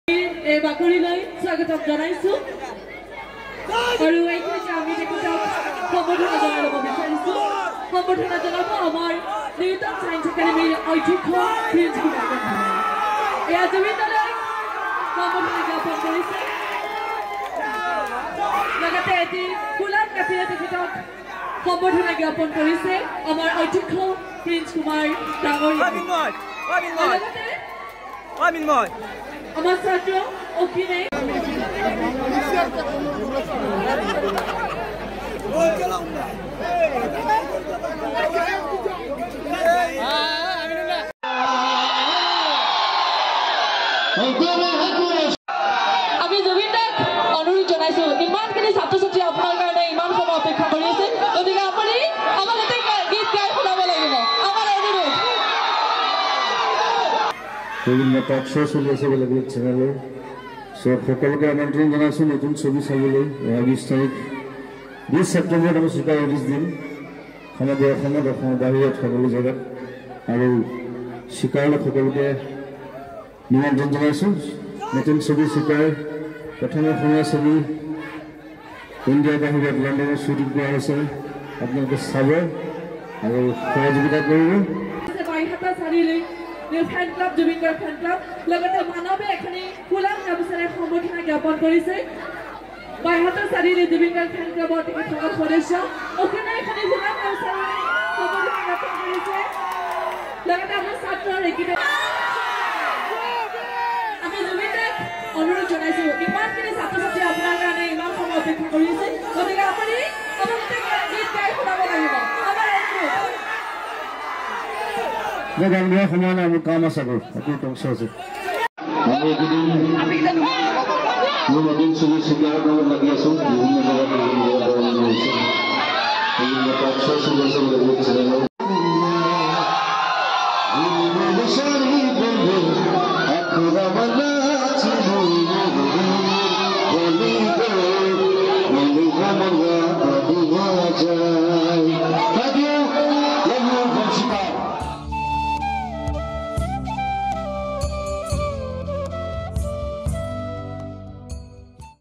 ▶বাইহাটা চাৰিআলিত জুবিন গাৰ্গ।
▶নিউটন  চায়েন্স একাডেমীৰ নৱাগত আদৰণি সভাত কন্ঠশিল্পী জুবিন গাৰ্গ।